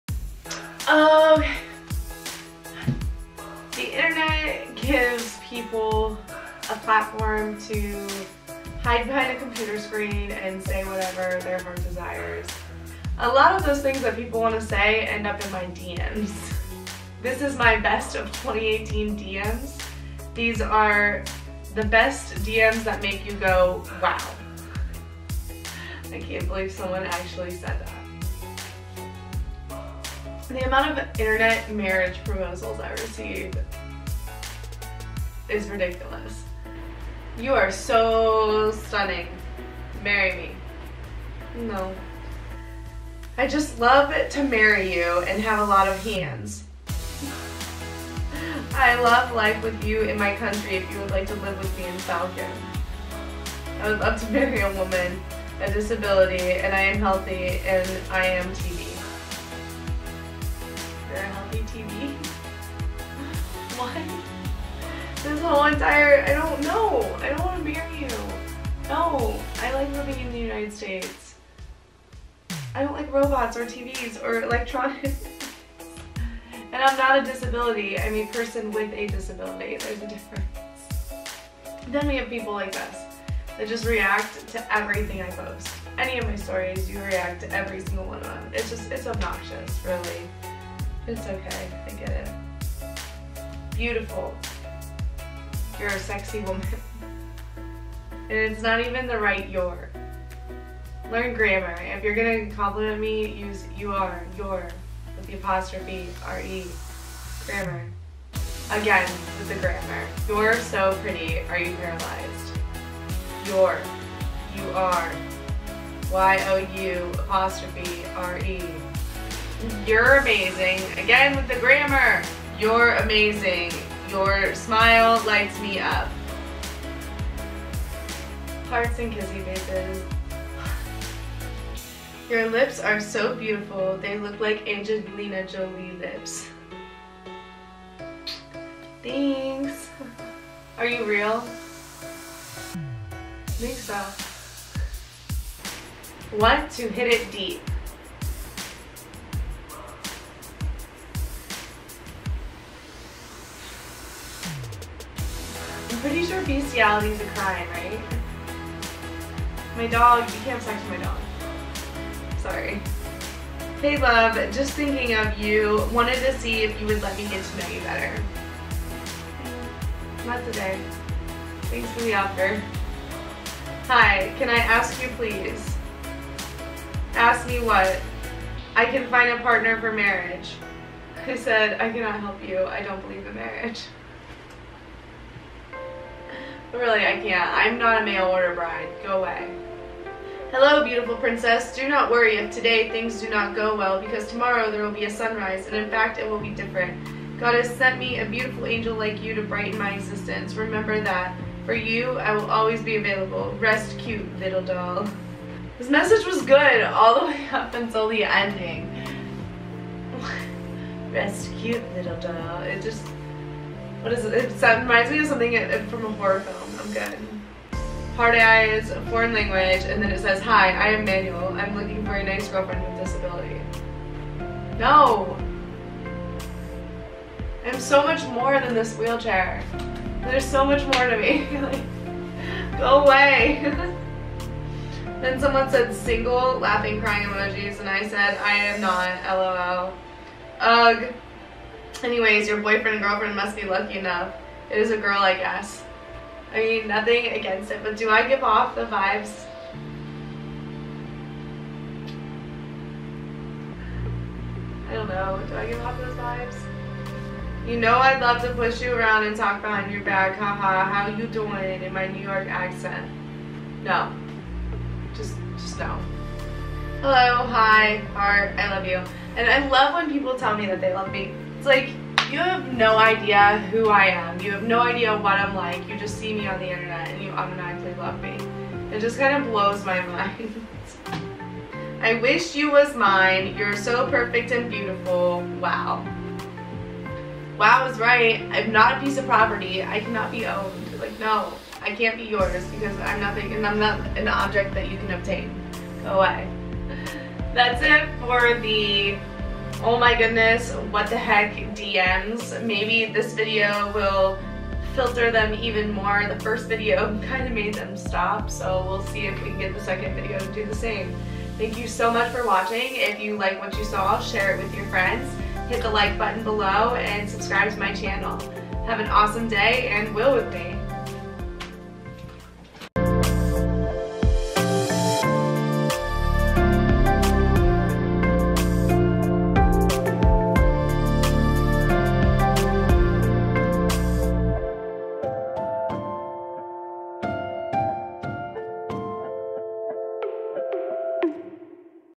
BEST OF THE WORST 2018 DM’s | EXTRA JESI STRACHAM
Back by popular request, Inside my Inbox with some of the craziest messages!

As a wheelchair user you become a commodity to a group of people we refer to as Devotees. Devotee's are attracted to wheelchair users, like most people are to someone's physical appearance.
I decided to share some of my personal favorite direct messages. I hope you guys enjoy as these as much as I enjoyed laughing at them!